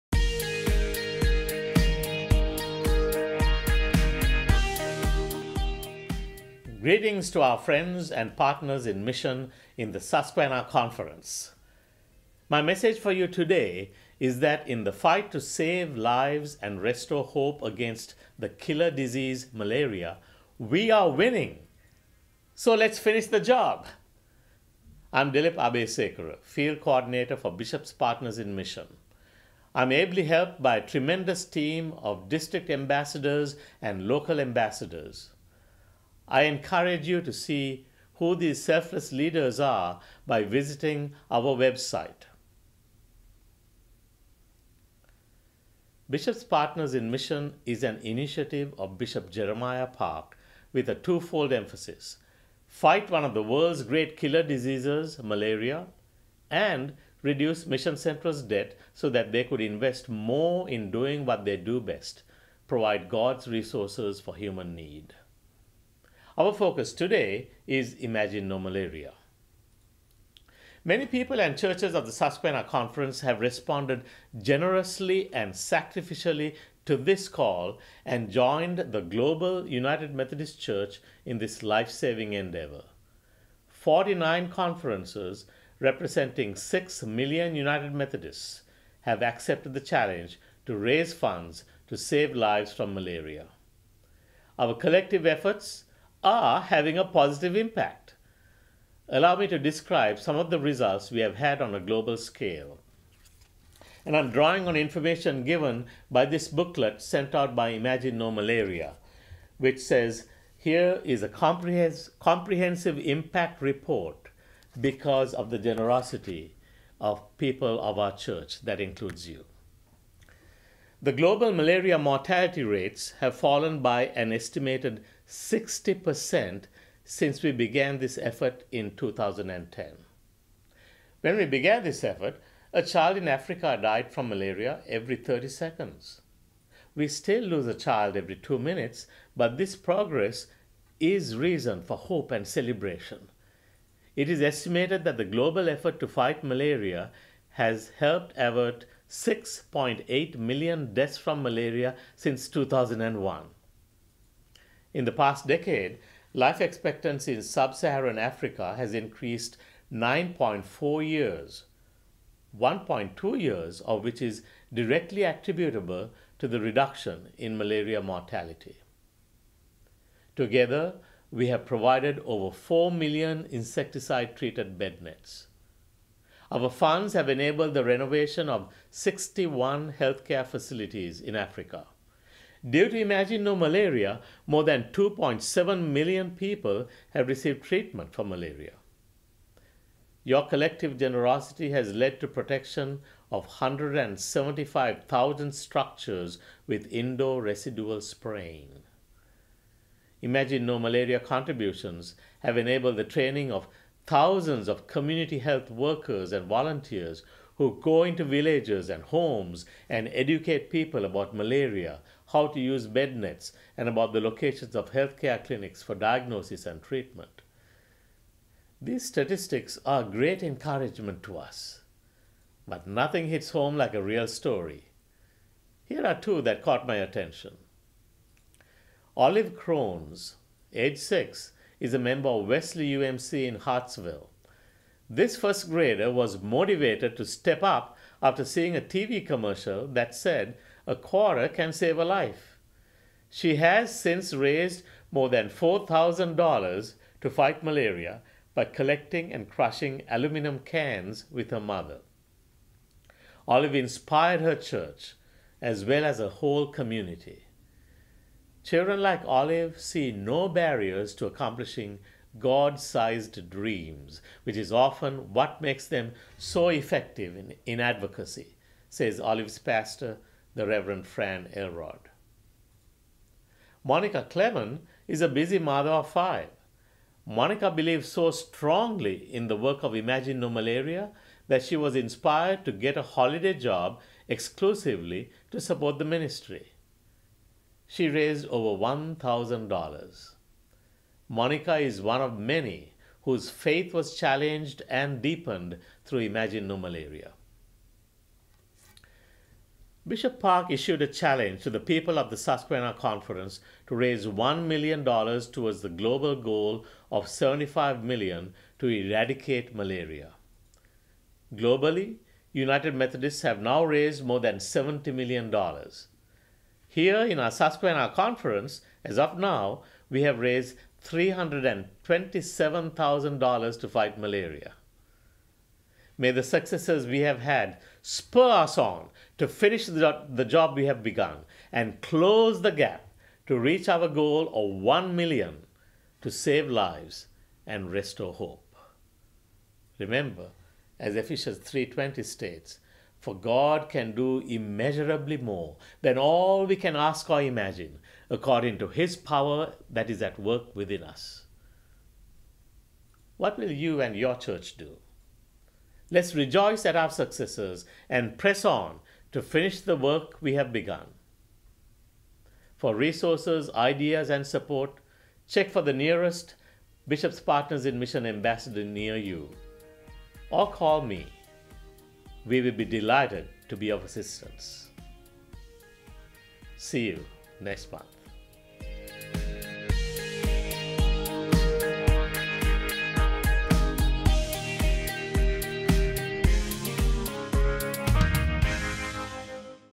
Imagine No Malaria Update: We are Winning!
Dr. Dilip Abayasekara gives an update on the positive impact of Imagine No Malaria globally, and locally.